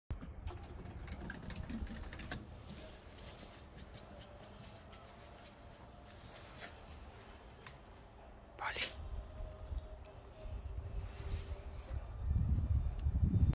Always Wipe Your Paws || ViralHog
Occurred on February 4, 2023 / Mössingen, Germany

"He always cleans his paws before entering the house. He's not trained. He does it on his own. He hates wet paws."